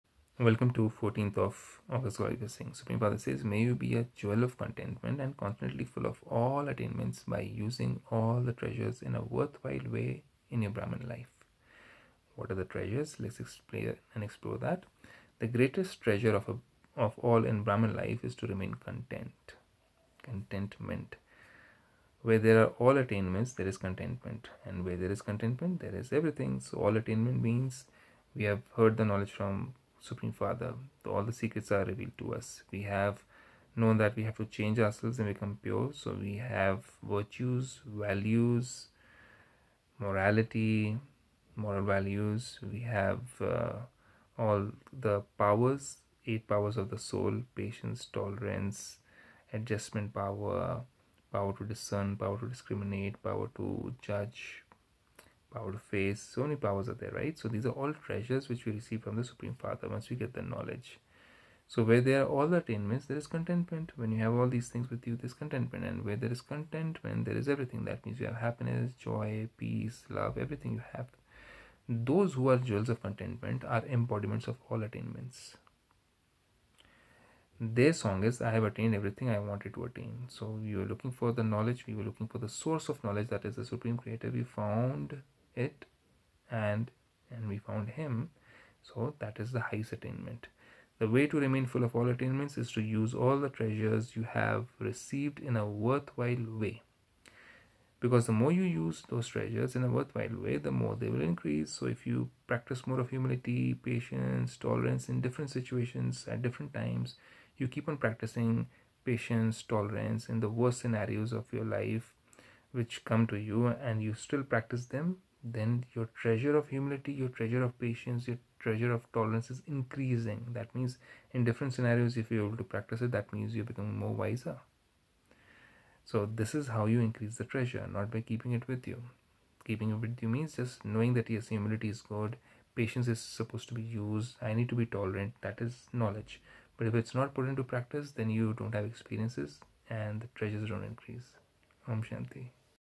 Be a jewel of contentment & constantly full of all attainments by using all the treasures in a ...
DAY 933 May you be a jewel of contentment and constantly full of all attainments by using all the treasures in a worthwhile way in your Brahmin life